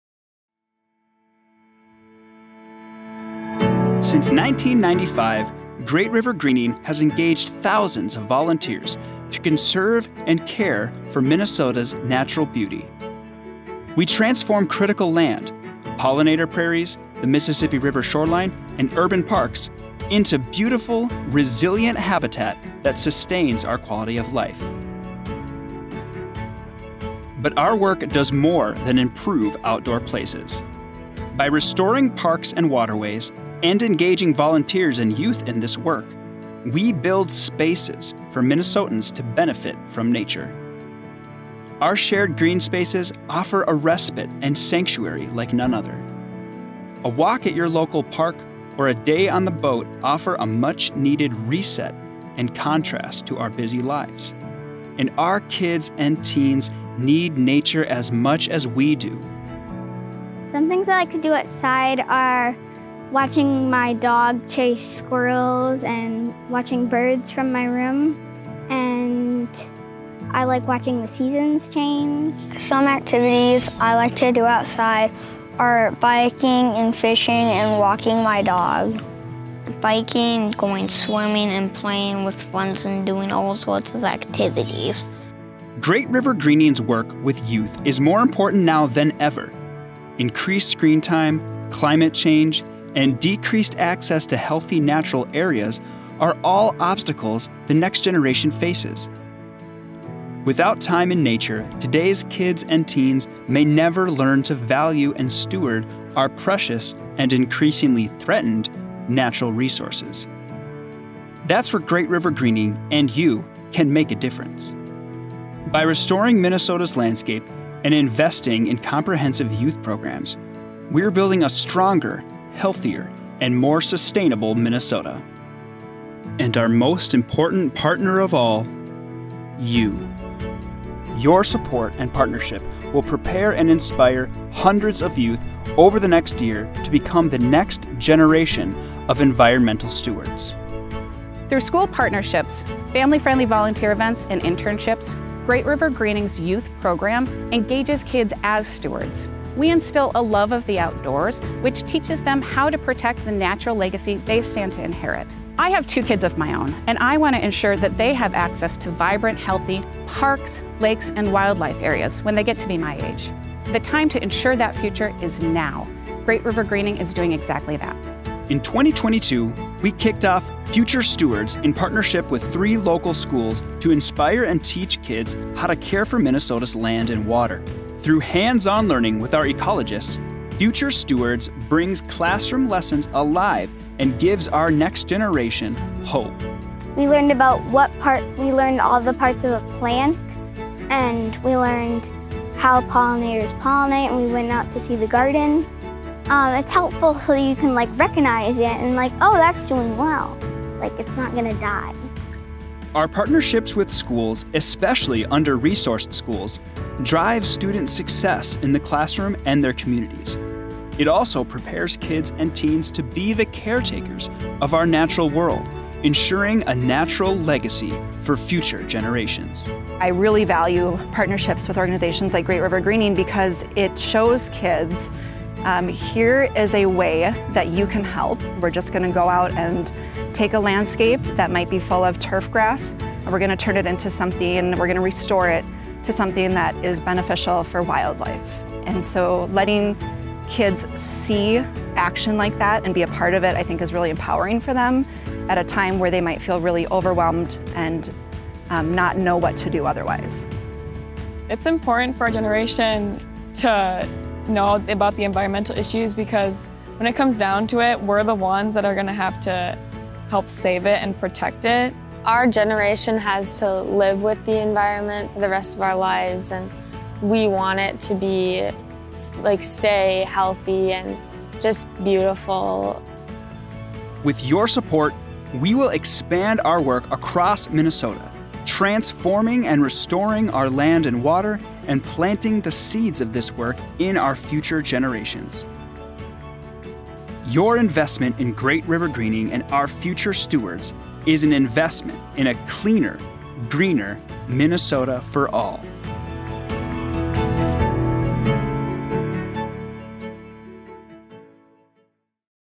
2022 Clean & Green Gala: Fund-a-Need
All Fund-a-Need proceeds from the 2022 Clean & Green Gala will benefit our Future Stewards and youth outdoor engagement initiatives. Future Stewards teaches youth the science of restoration and the benefits of spending time in nature. Through hands-on nature-based learning at their schools, this initiative increases participation in outdoor activities that drive student success in school, at home, and in their communities. Learn more about Great River Greening's Future Stewards initiative

Additional videography by Brett Randolph.